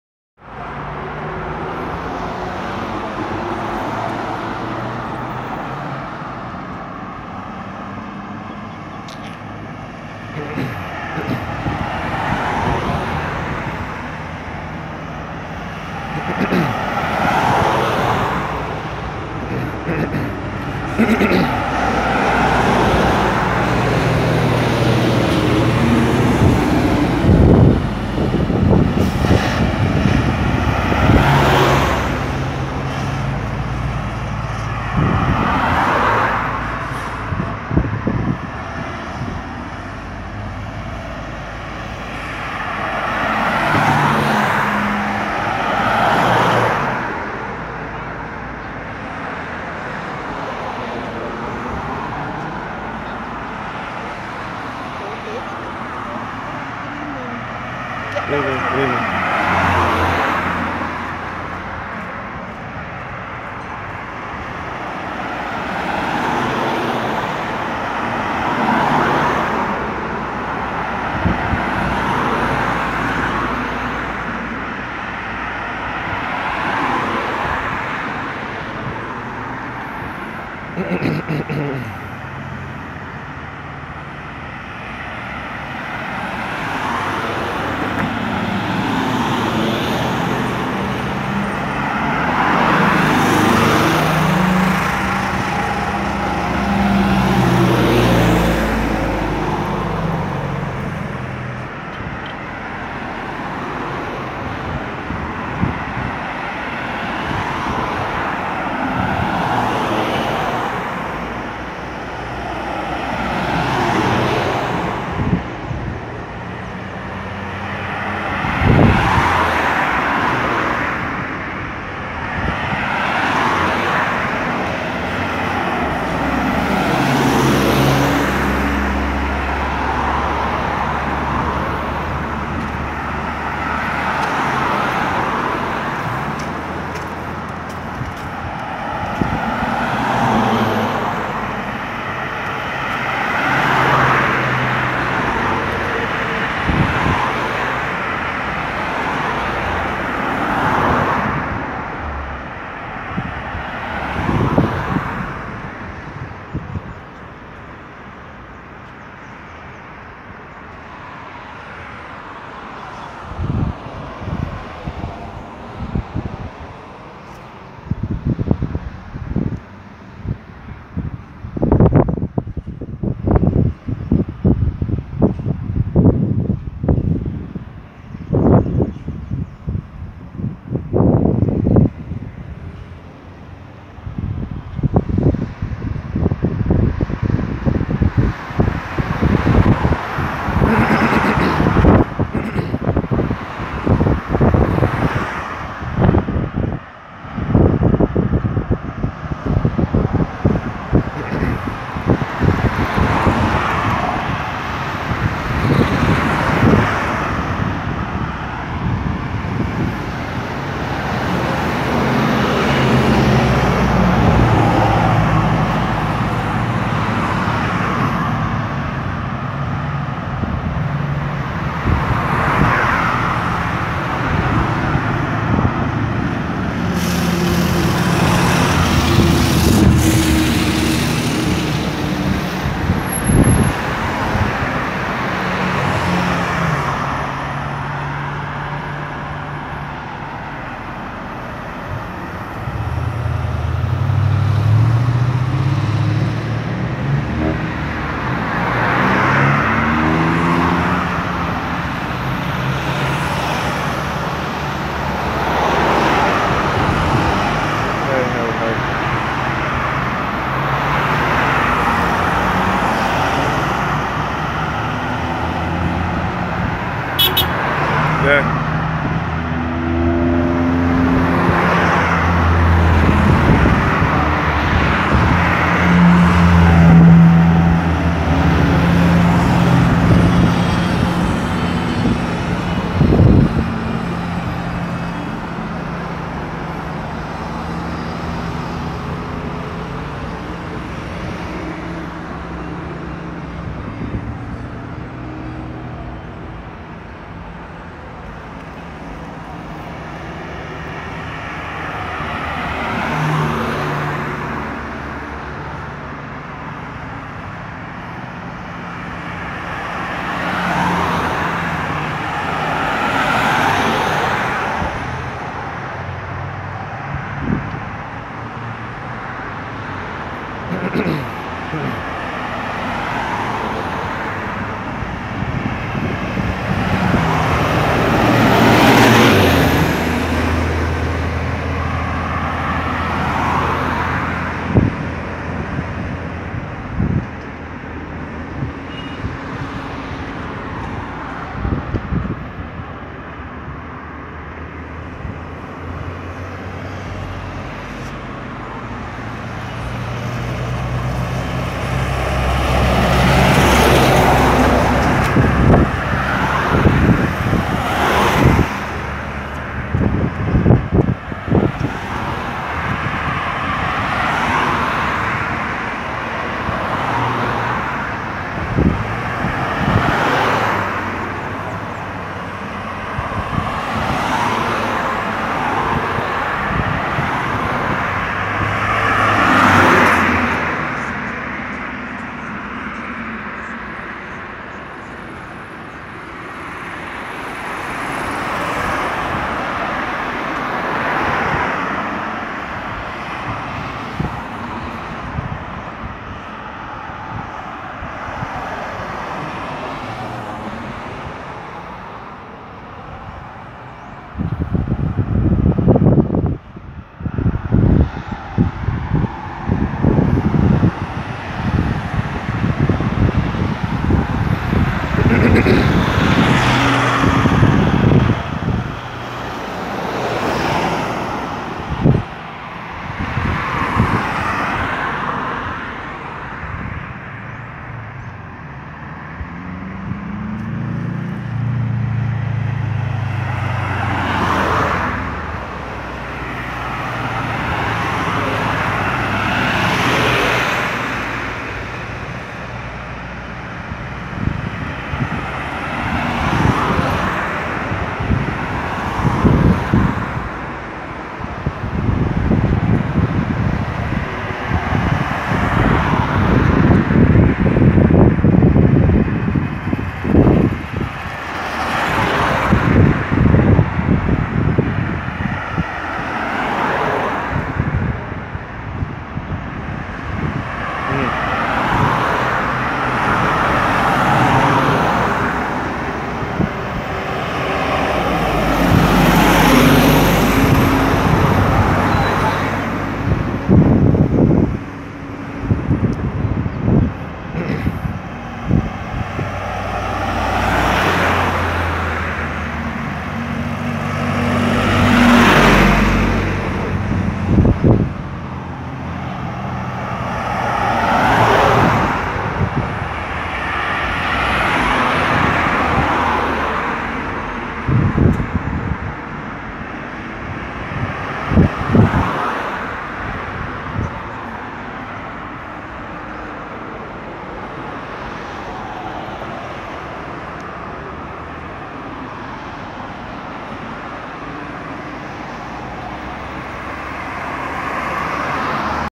Warrens Business & Commercial Area Barbados🇧🇧(city away from city)
This is the section of the Warrens area , north of the D'Arcy Scott Roundabout , (adjacent to Simpson Motors) which commences the Ronald Mapp Highway (Highway 2A) which would conclude at Mile & A Quarter in St. Peter.